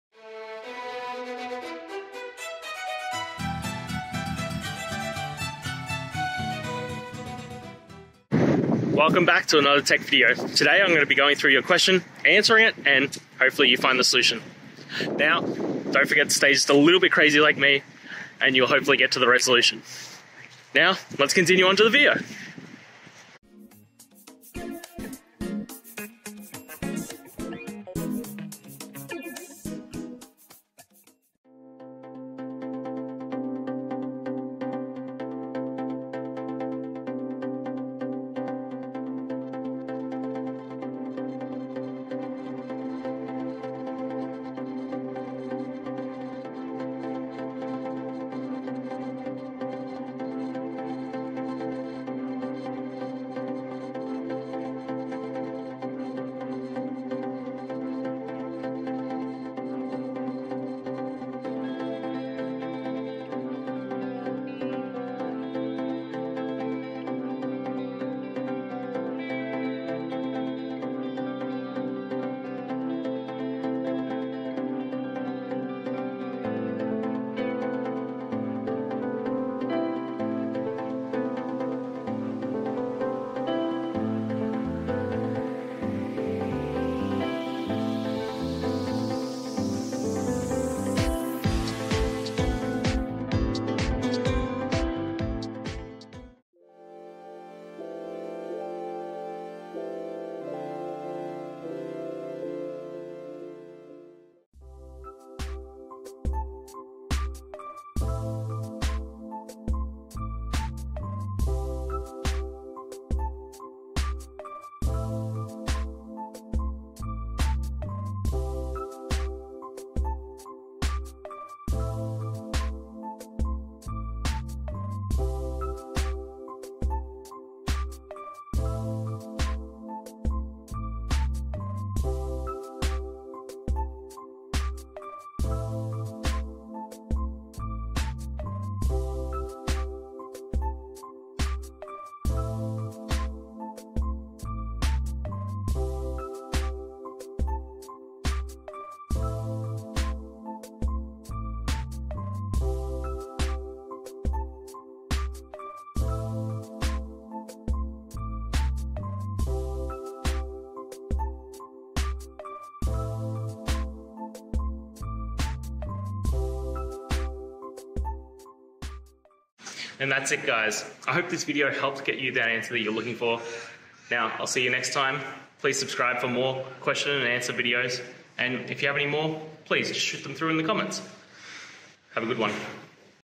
How to Check if a Timezone Name is Valid in JavaScript: A Complete Guide
In this video, we’ll explore the essential topic of validating timezone names in JavaScript. As applications become increasingly global, ensuring that your code correctly handles different timezones is crucial for providing accurate date and time information. Join us as we walk through various methods and best practices to check if a timezone name is valid, equipping you with the knowledge to enhance your JavaScript projects.

Today's Topic: How to Check if a Timezone Name is Valid in JavaScript: A Complete Guide

Related to